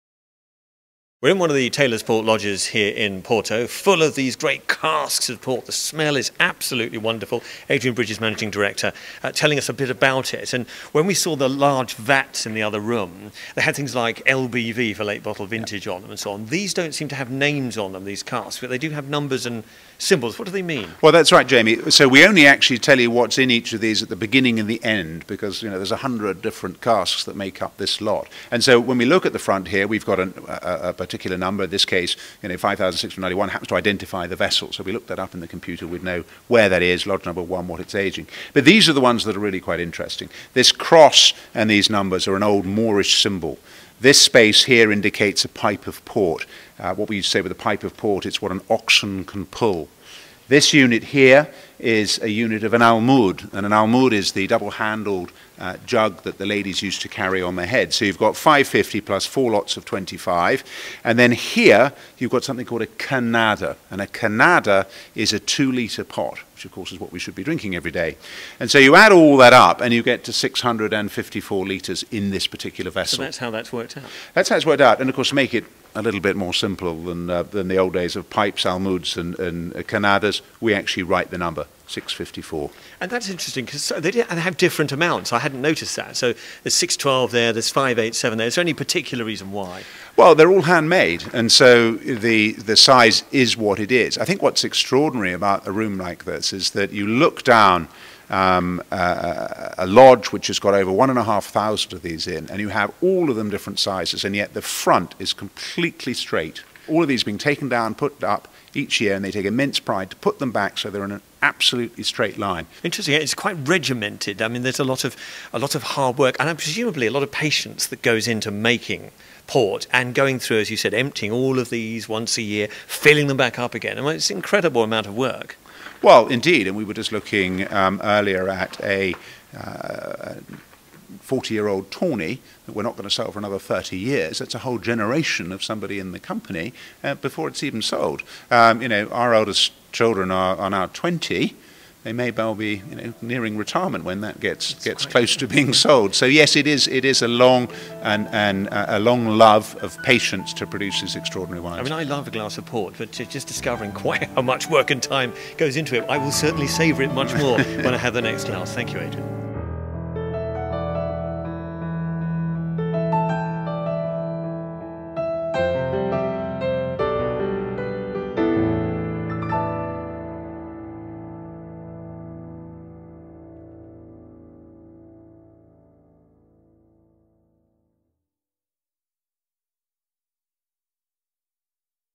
Taylor's Port with Jazz FM - Markings on a cask
A journey into the infamous Taylor's Port Cellars in Porto and the dedication and craftsmanship that goes into making these wonderful  fortified wines.